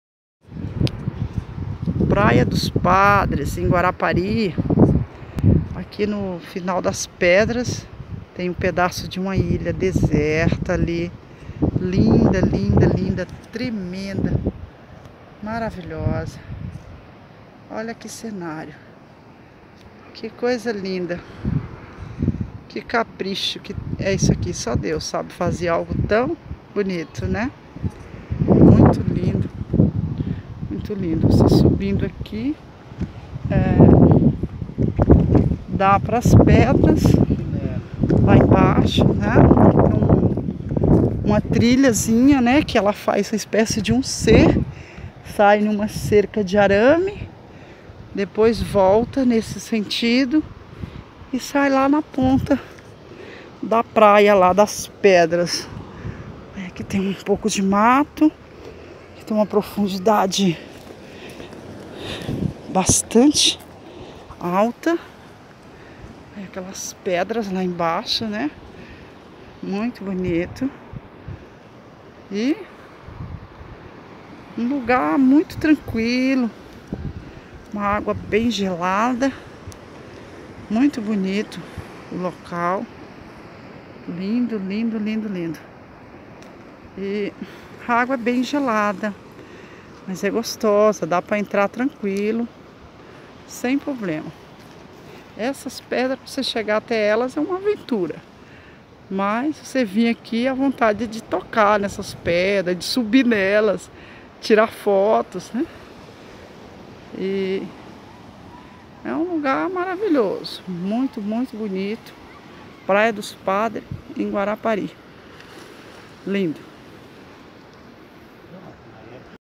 PRAIA Dos padres em Guarapari
vem ver que praia linda praia dos padres Guarapari ES com lindas pedras enormes#praiadospadresemguarapari#vemcomadonnamaria